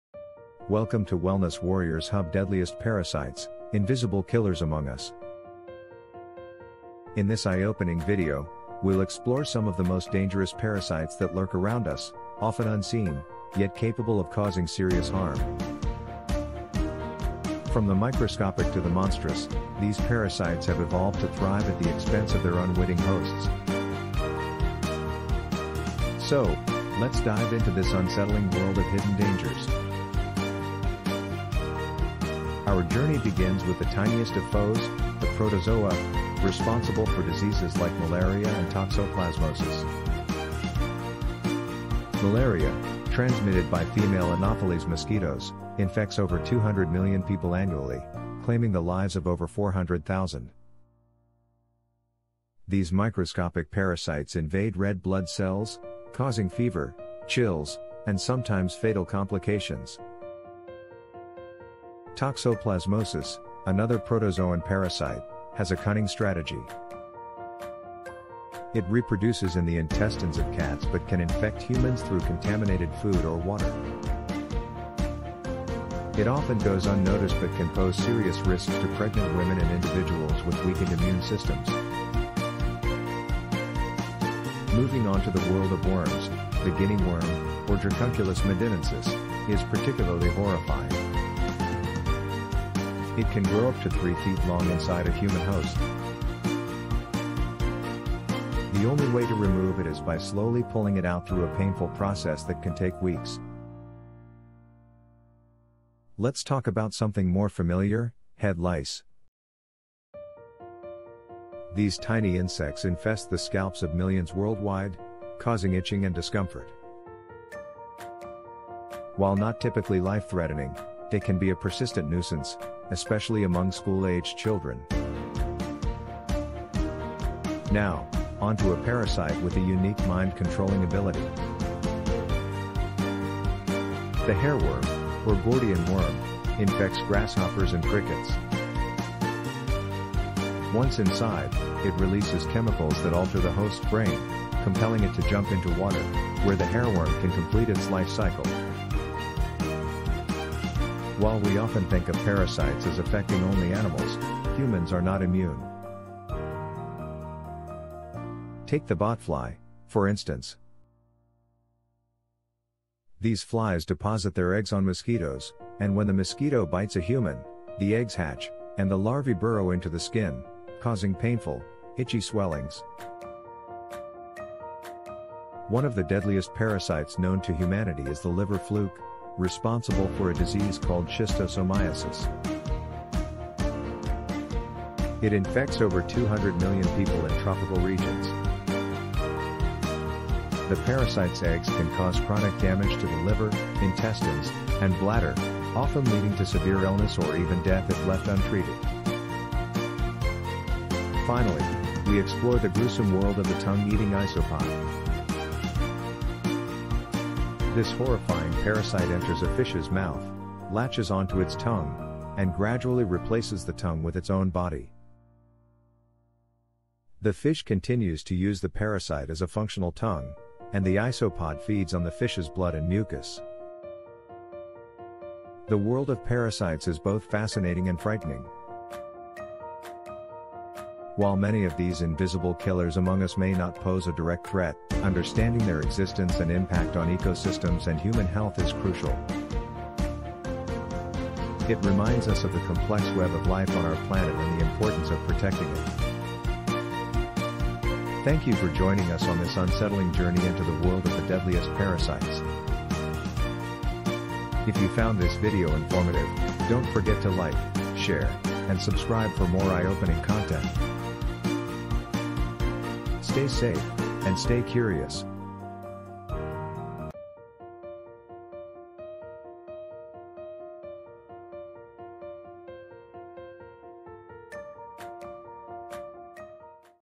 Deadliest Parasites: Invisible Killers Among Us
"Deadliest Parasites: Invisible Killers Among Us." In this eye-opening video, we'll explore some of the most dangerous parasites that lurk around us, often unseen, yet capable of causing serious harm. From the microscopic to the monstrous, these parasites have evolved to thrive at the expense of their unwitting hosts. So, let's dive into this unsettling world of hidden dangers.